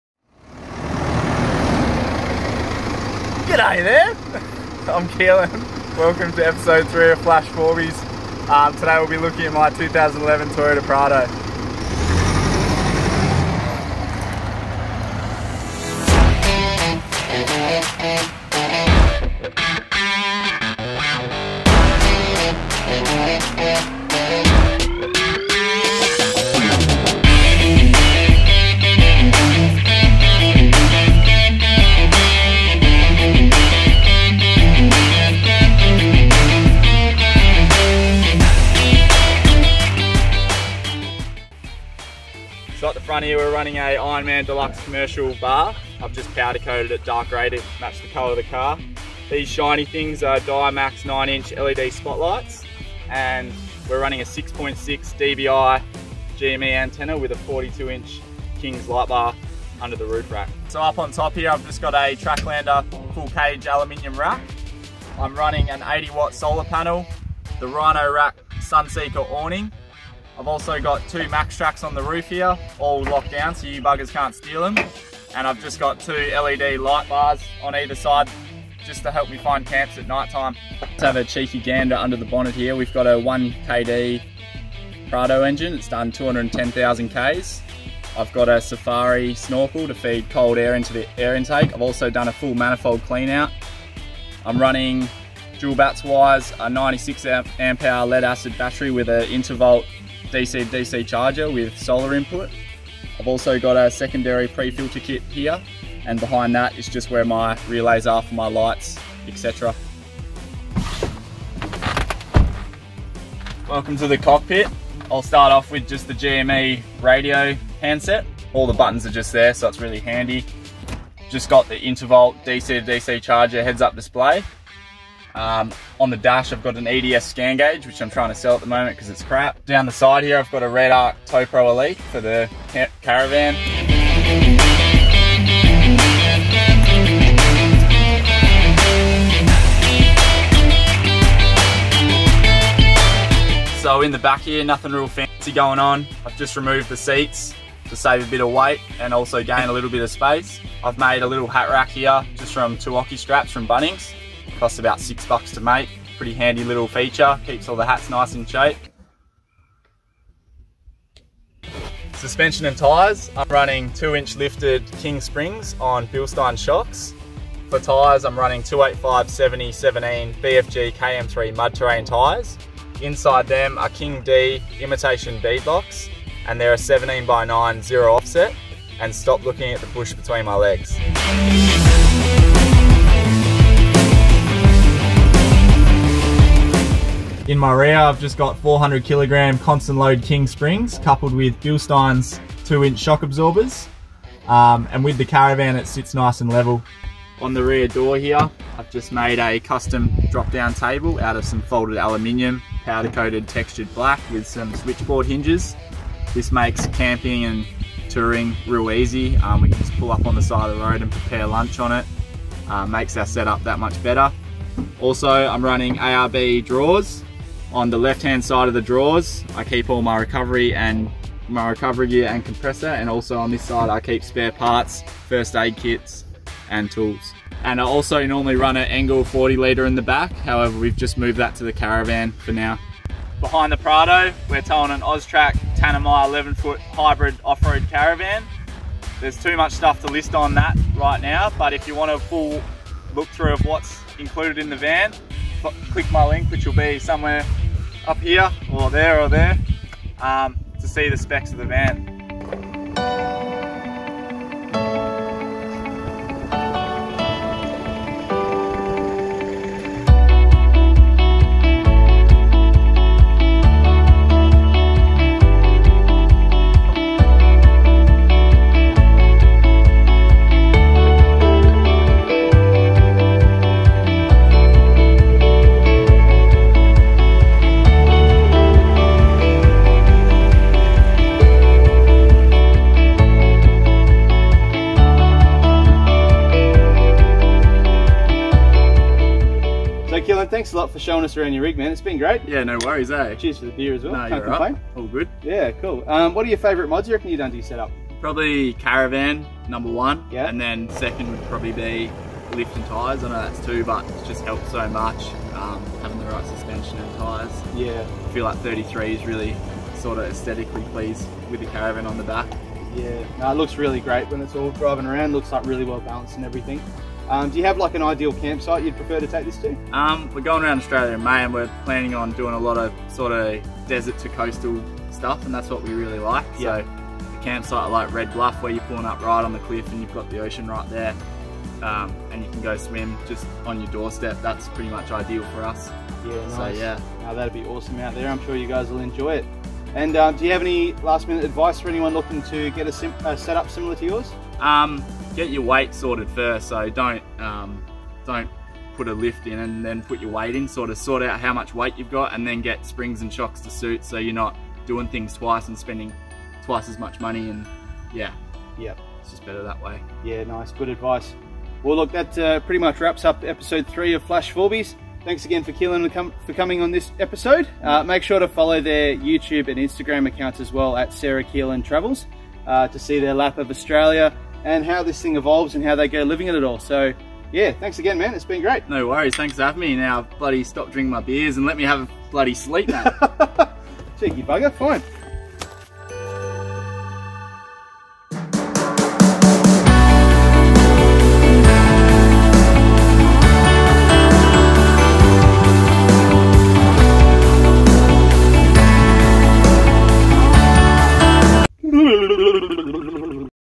FLASH FOURBYS | Episode 3: Sarah & Keelan's Prado 150
In this series we take a look at some of the best kitted out 4WDs in Australia, with emphasis on the mods and custom works that make them stand out from the crowd.
The third rig to join our series is special guest Keelan and his 150 Series 1KD Prado, setup to take him and his partner around Australia with their flash Oztrack Tanami X11 Offroad caravan.
Be sure to follow them on the socials @SarahandKeelanTravels to see more of their adventures!